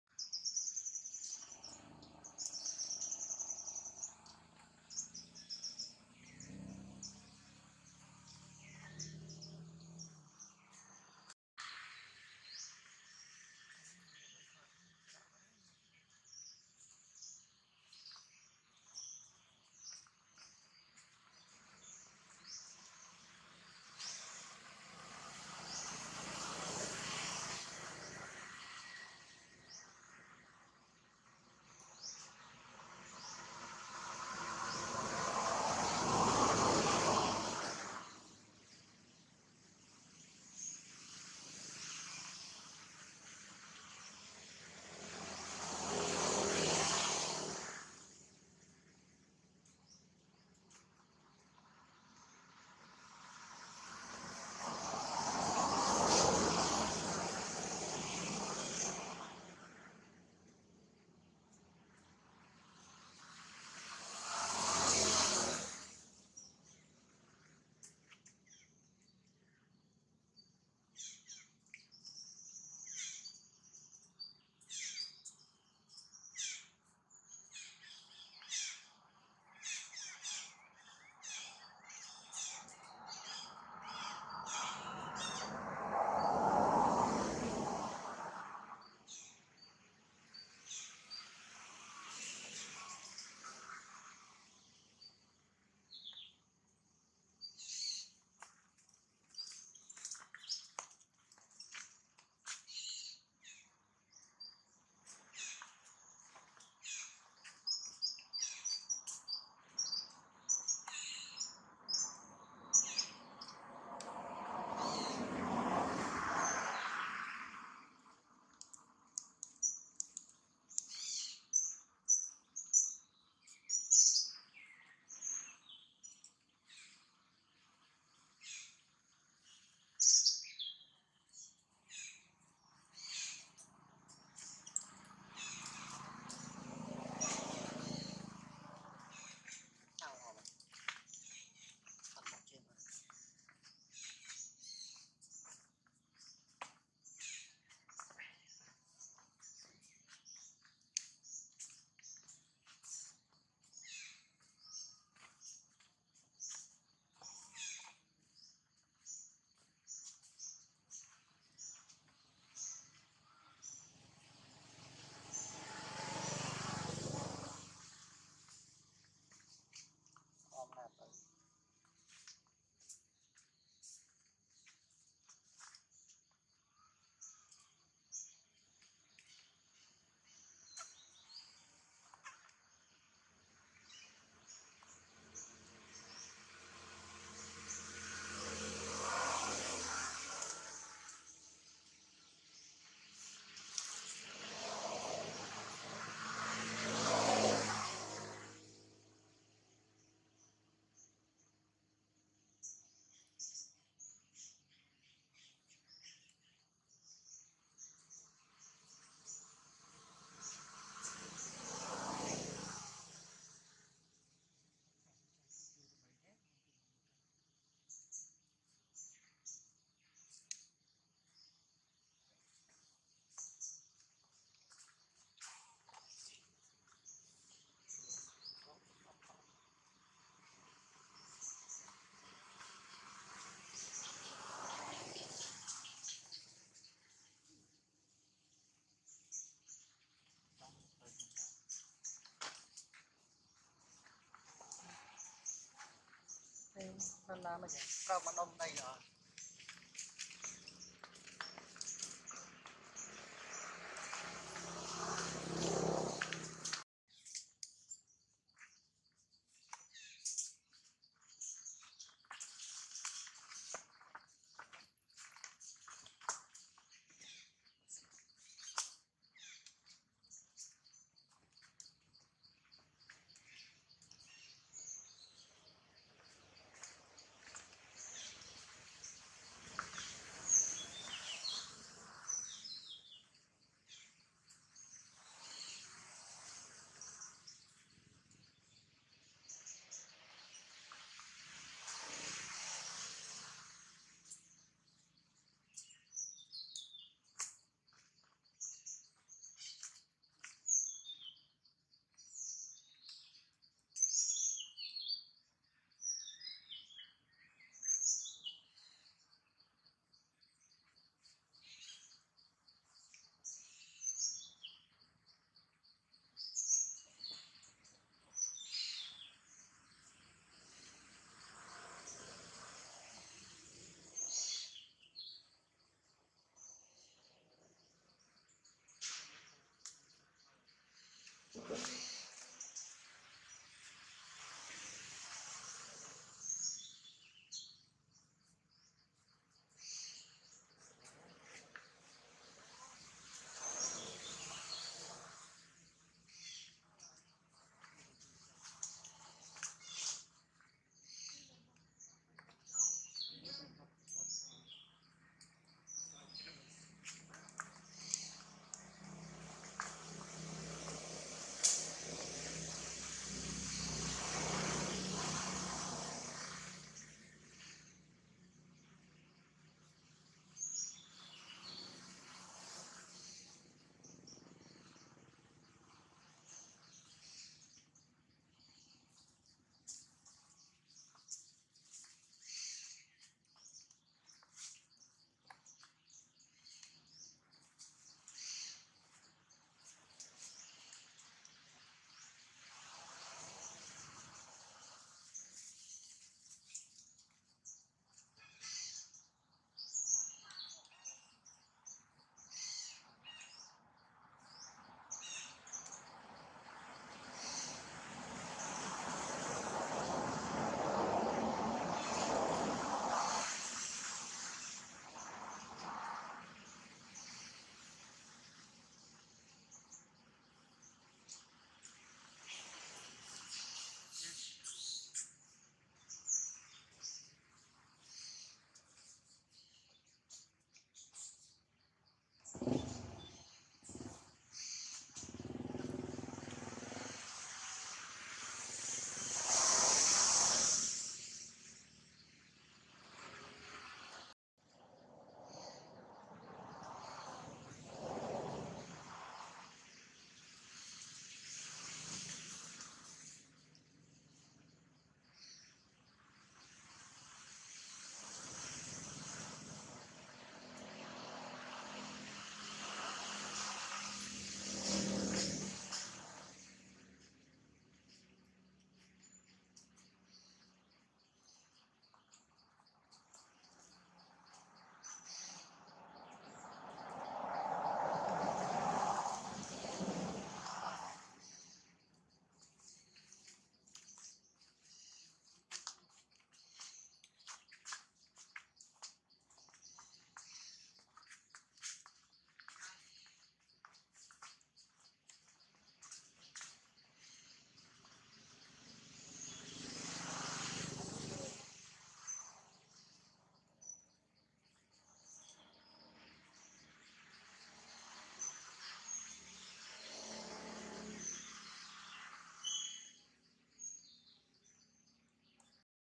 I know you feeling hurt Jane, Very hard for Jane try to give birth baby, Jane show us look so hurt
Everyday Monkey